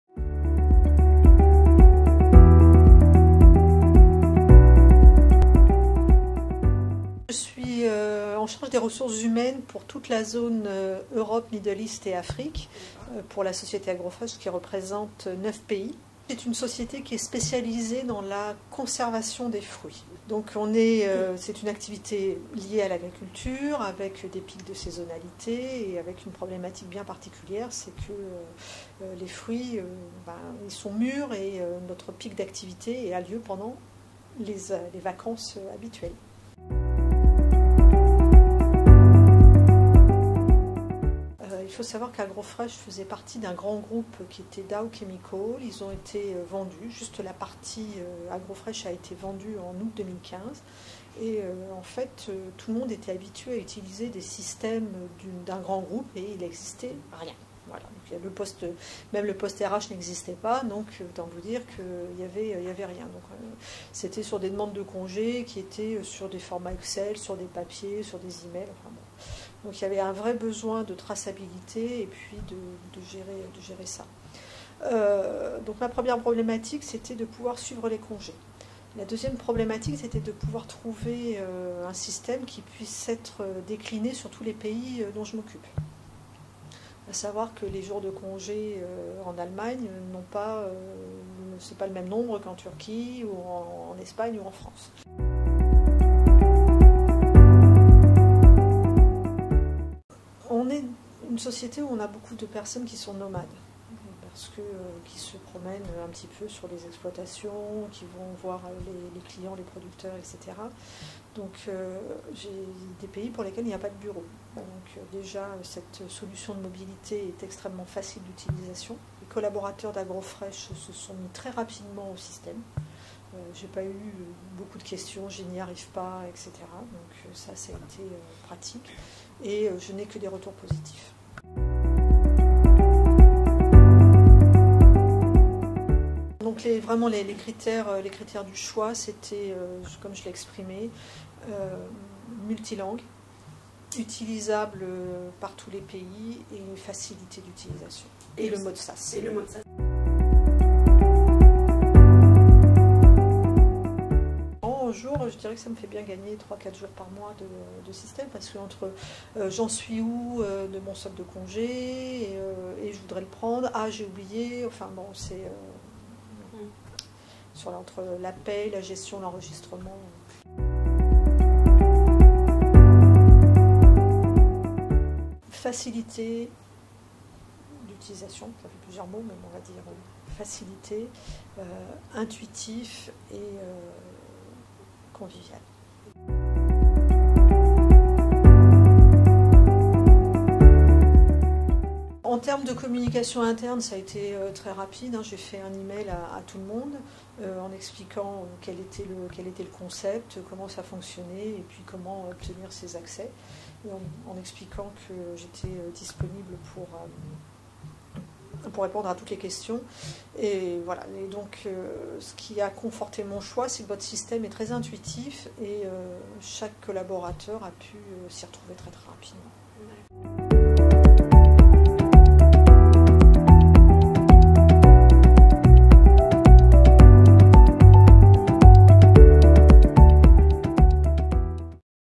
Agrofresh utilise la solution Eurécia pour gérer les congés à l'international
Agrofresh spécialiste dans la conservation des fruits, est client d’Eurécia depuis plus d’un an.
Aujourd’hui, l’entreprise compte 78 collaborateurs répartis dans neuf pays européens.
Suite à la sortie d’un grand groupe, Agrofresh a dû se structurer en créant un service RH et en trouvant un outil leur permettant de gérer les congés, déclinable à l’international.
La société a donc fait appel à Eurécia pour le suivi des congés et absences de leurs employés qui sont majoritairement nomades.
La rédaction est allée à la rencontre de Fabienne Sixdeniers, directrice des ressources humaines Europe. Ce que l’on retiendra de notre entretien c’est qu’Agrofresh gagne trois à quatre jours par mois grâce à la solution Eurécia.
Retour en vidéo.

En savoir plus sur le témoignage de notre client Agrofresh rendez-vous sur notre blog